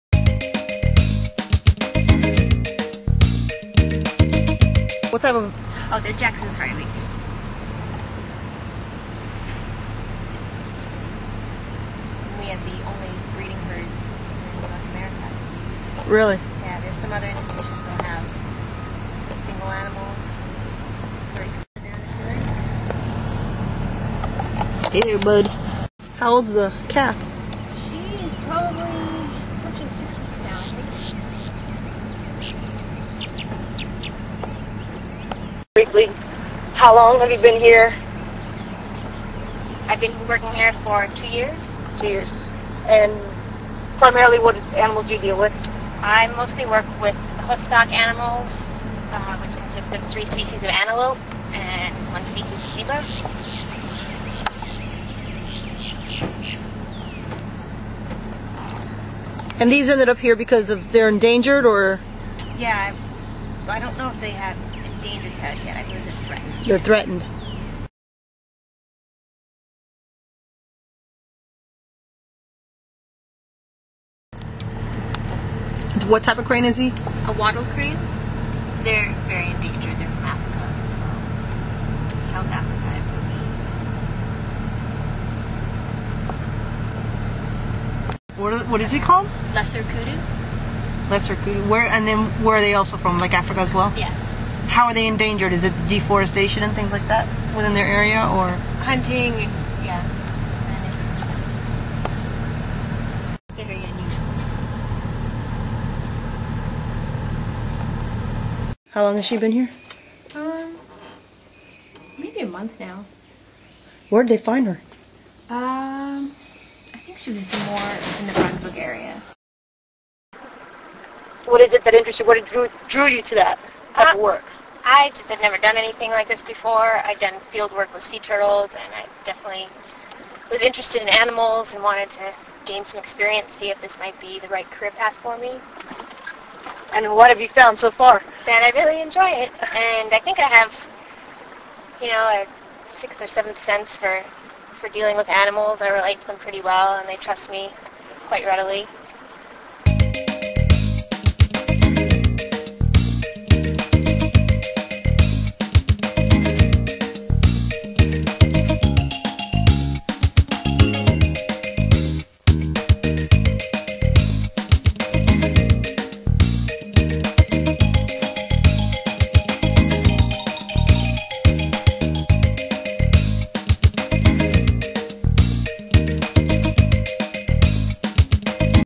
St. Catherines Island Wildlife survival center: The hoofstock animals
antelopes, hartebeest, zebras OH MY